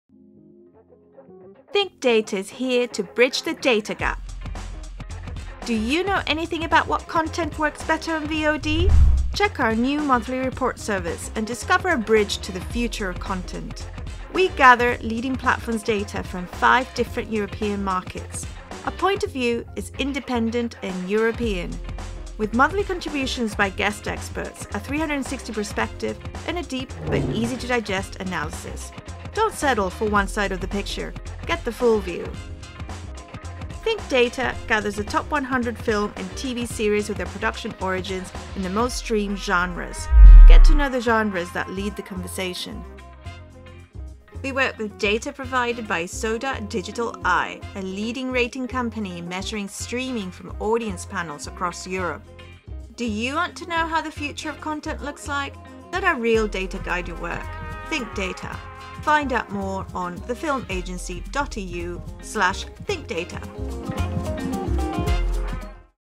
Think Data - Trailer
We’re launching a new product: THINK DATA: BRIDGING THE DATA GAP. This will be a unique set of accessible, synoptic, and analytical data reports from the perspective of the independent audiovisual tissue. Our first product is a monthly subscription of analytical reports of SVOD data. Our subscribers will receive a monthly newsletter with the top 100 movies and series, the most-streamed genres, and production country origins on the SVOD platforms in strategical territories. Moreover, they will join a short webinar where we will analyze and discuss the most important findings. This valuable data is provided by our partner SODA – iDigital, a leading UK-based data rating company holding a panel of 5K SVOD viewers based in Italy, France, Germany, Spain, and the UK, and measuring all streaming on Netflix and Amazon. SODA is soon expanding to other territories and platforms that we will add to future monthly reports. These quick charts will allow the reader to take the temperature of the market at a glance. Digging further, in the analytical texts, we take a closer look at independent titles, flagship genres, and European content. We intend to go beyond the usual blockbuster mainstream performance data, to better understand what lies below the main hits and what can converge as an opportunity for our clients